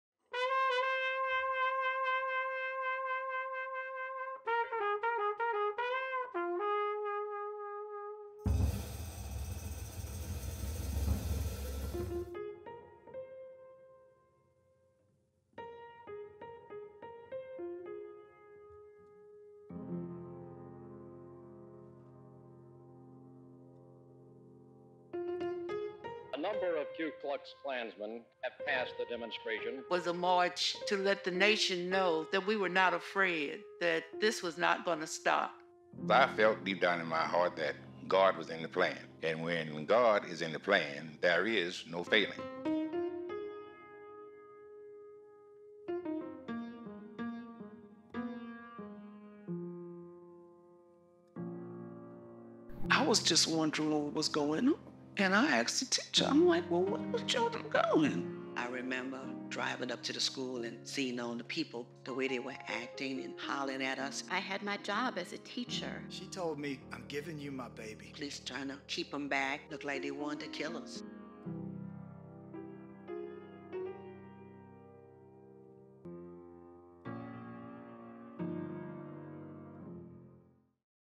Louisiana Civil Rights Museum's 'Dream Cube' Trailer
The new Louisiana Civil Rights Museum launched its inaugural exhibition, inviting visitors to enter history and live through the harrowing Civil Rights journeys that shaped our nation. The exhibit concludes with the state-of-the-art 'Dream Cube,' an immersive environment designed by Local Projects, led by experiential studio Solomon Group, and implemented by creative studio The-Artery using projection mapping and motion detection.

In the Dream Cube, visitors experience the longest civil rights march and join the first Black students to enter desegregated elementary schools amidst fierce protest and disapproval.

The-Artery used ZED 2 Stereo cameras to combine advanced depth sensing with spatial AI to track body movements that activate stories from the protestors behind fiery demonstrations or those students, teachers, and U.S. Marshals who braved crowds during an elementary school’s desegregation. Each immersive film pairs with original audio interviews from central figures and original music by Antfood.

The creative studio also handled the transition of the still photographs, first encountered by audiences, into realistic and immersive video, using tools including Unreal Engine, Blender, Zbrush, Maya, and Autodesk 3ds Max. To achieve this seamless look, the team expanded the environment of each photograph with video assets as an alternative to stretching the picture itself, retaining the photo subjects front-and-center and keeping them the point of focus, even as animated crowds of people envelop each visitor.